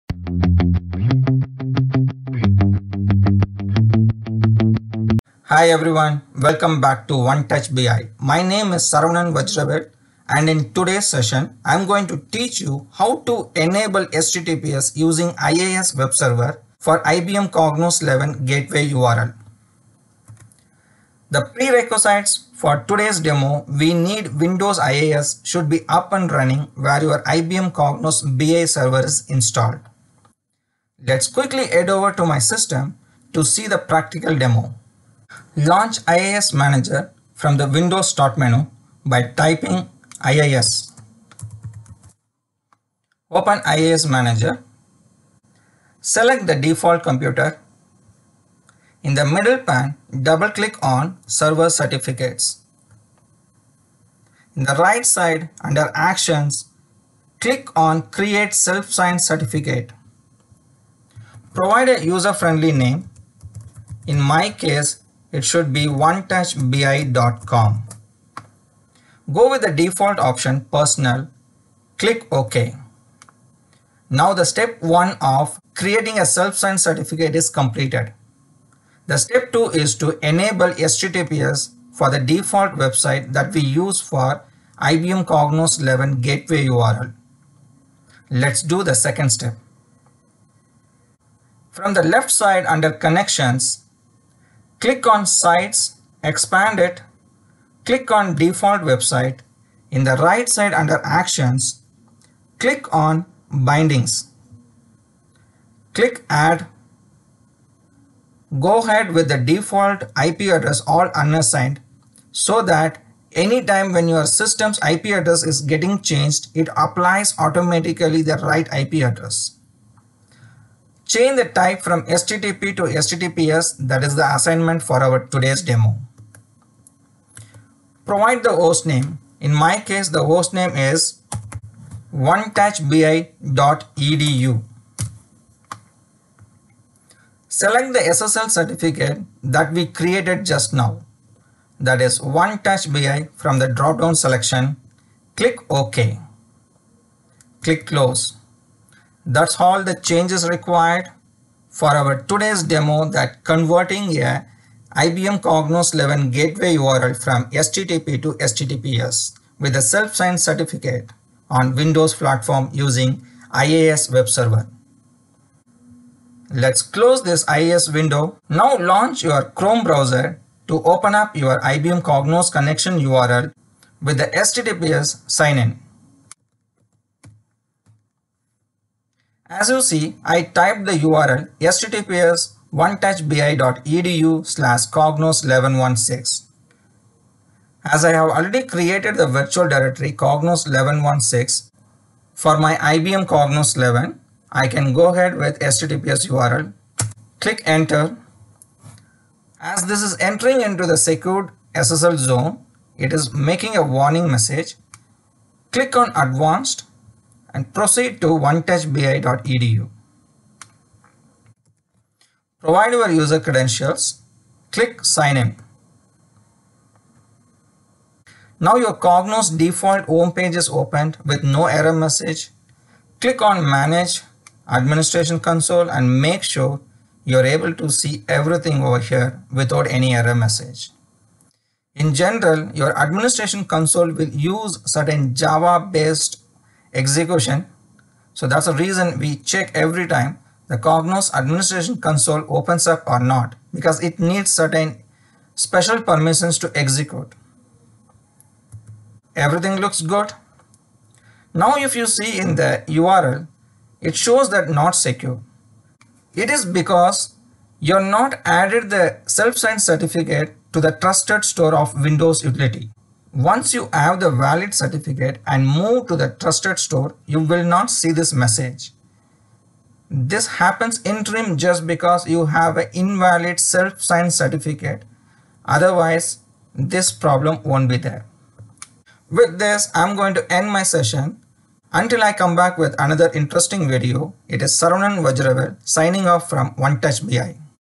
IBM Cognos 11 - Admin | Enable HTTPS using IIS Server with Self Signed Certificate - Tutorial 4
Welcome to the knowledge base portal It's the platform to connect all BI Associates under one umbrella.

IBM Cognos 11 - Enable HTTPS using Windows IIS Server with Self Signed Certificate - Tutorial 4